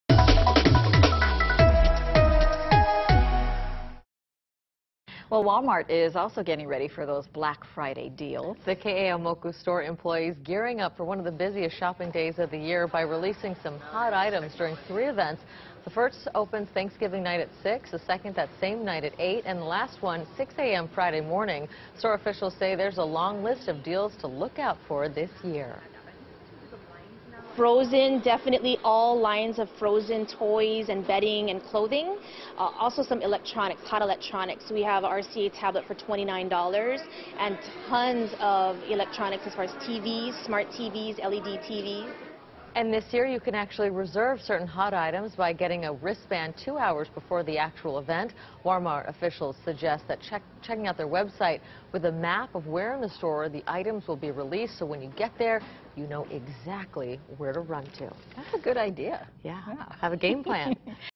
Walmart prepares for Black Friday sales
Keeaumoku store employees gearing up for one of the busiest shopping days of the year by releasing hot items during three shopping events.